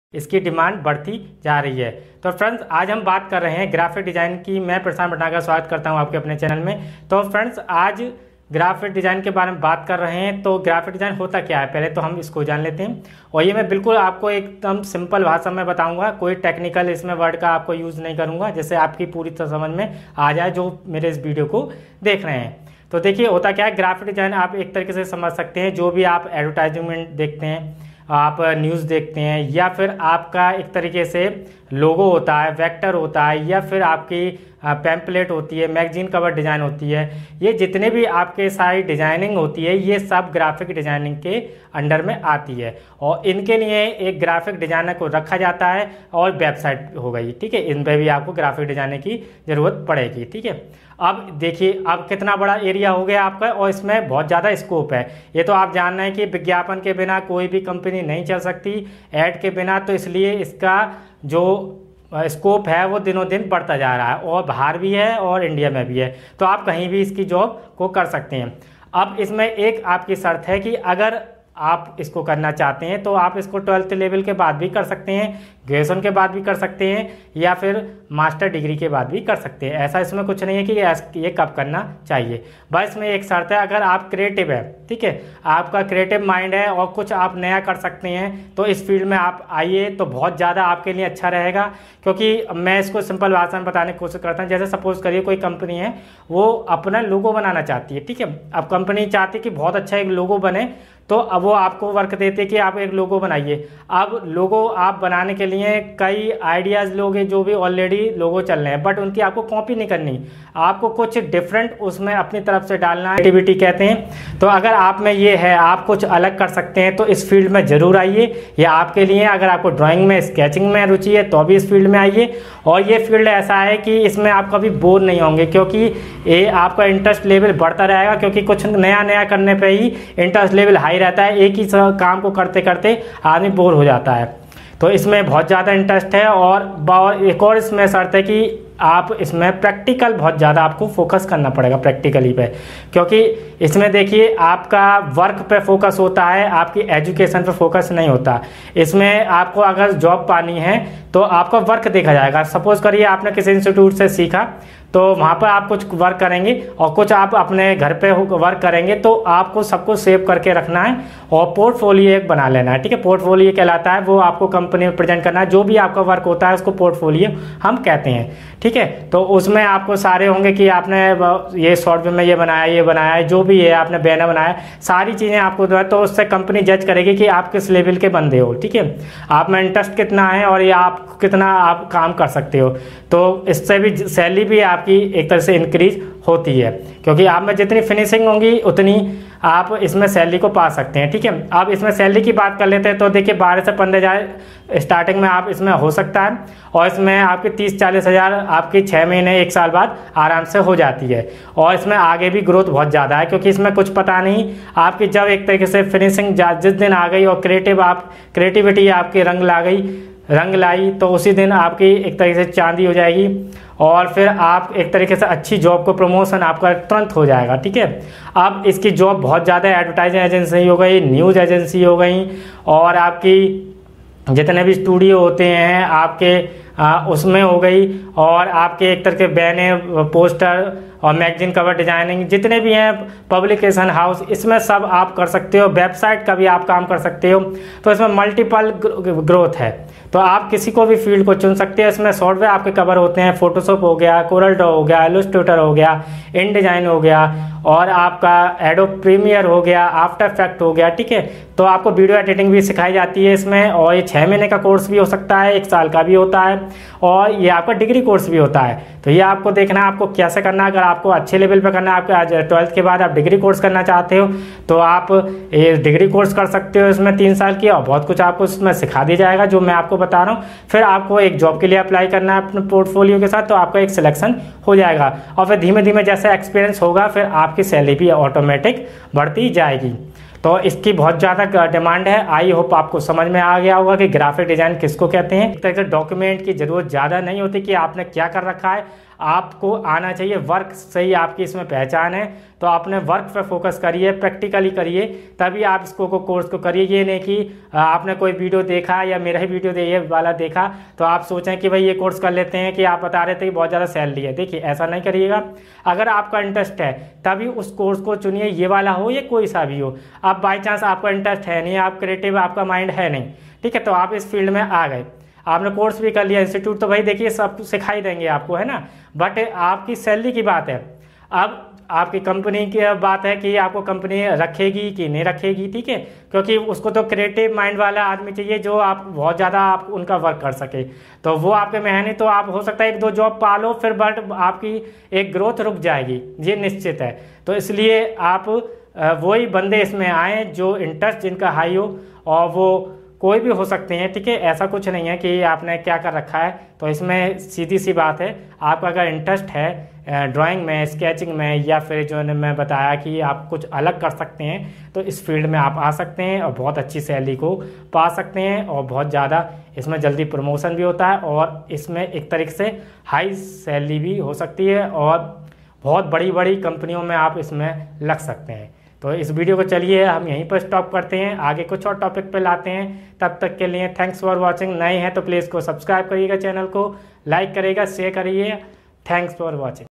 Graphic Design Career In India 2020 | Graphic Design Course | graphic design in hindi
hello freinds is video me aap graphic design me kaise apna career banaye kya salary hogi kon ye course kar sakta hai etc bataya hai